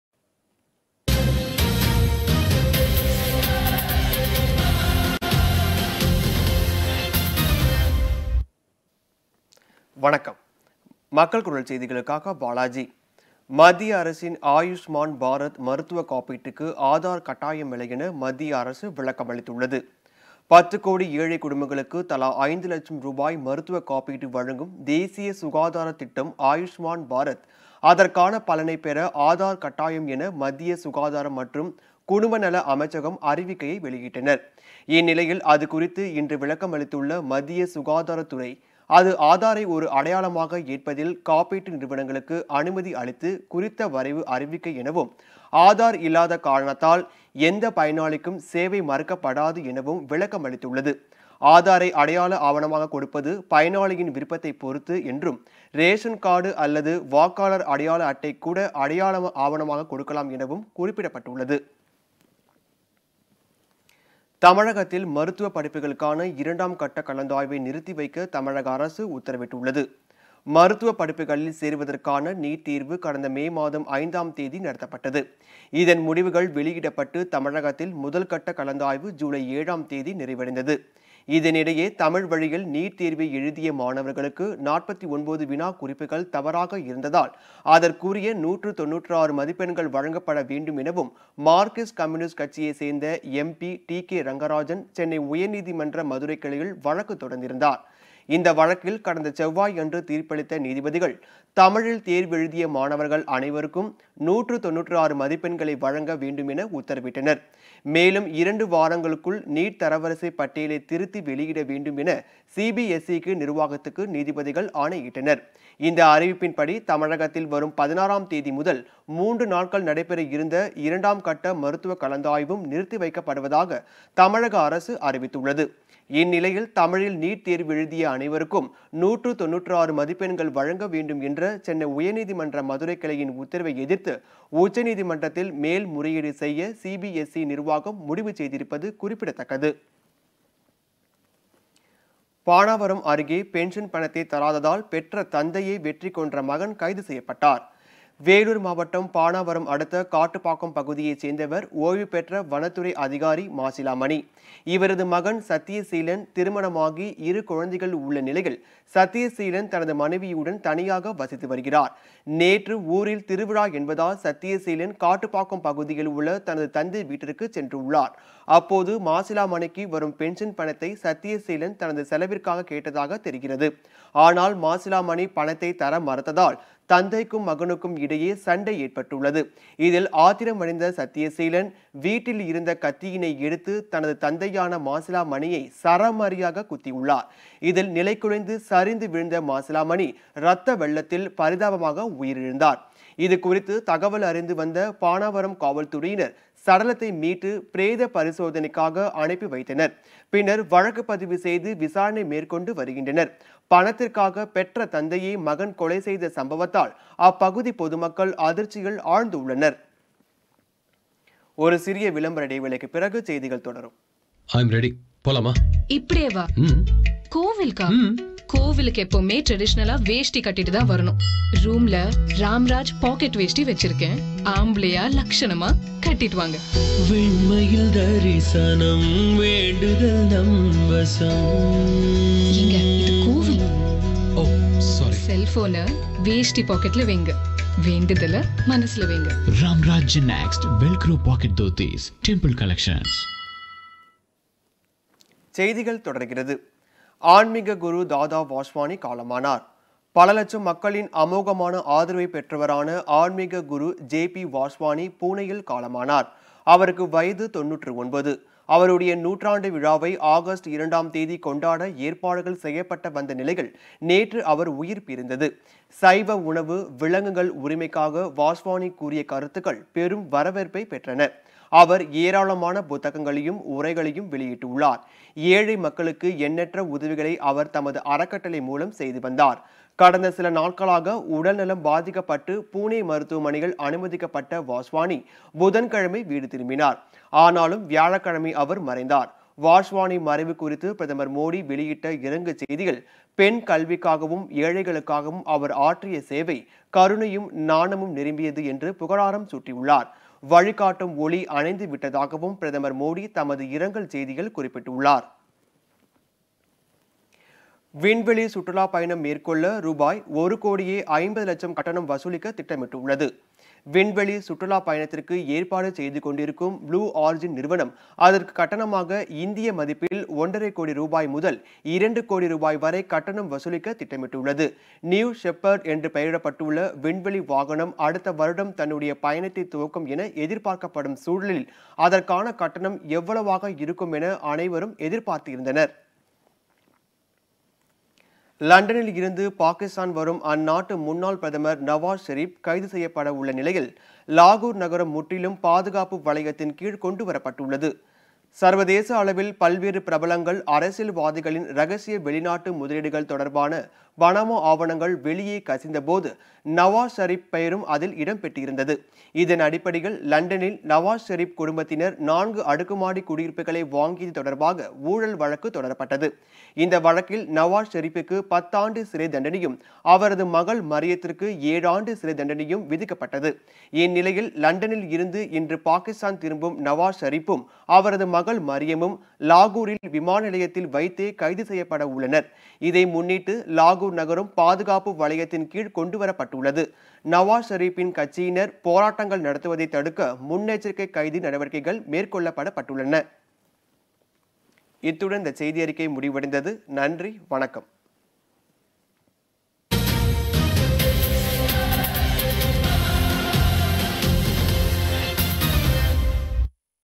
Rs 1.5 crore for the space tour | விண்வெளி சுற்றுலாப்பயணம் மேற்கொள்ள ரூ.1.5 கோடி கட்டணம்
விண்வெளி சுற்றுலாப் பயணத்திற்கு ஏற்பாடு செய்துகொண்டிருக்கும் Blue Origin நிறுவனம், அதற்கு கட்டணமாக இந்திய மதிப்பில் ஒன்றரை கோடி ரூபாய் முதல் 2 கோடி ரூபாய் வரை கட்டணம் வசூலிக்க திட்டமிட்டுள்ளது.New Shepard என்று பெயரிடப்பட்டுள்ள விண்வெளி வாகனம் அடுத்தவருடம் தன்னுடைய பயணத்தை துவக்கும் என எதிர்பார்க்கப்படும் சூழலில், அதற்கான கட்டணம் எவ்வளவாக இருக்குமென அனைவரும் எதிர்பார்த்திருந்தனர்.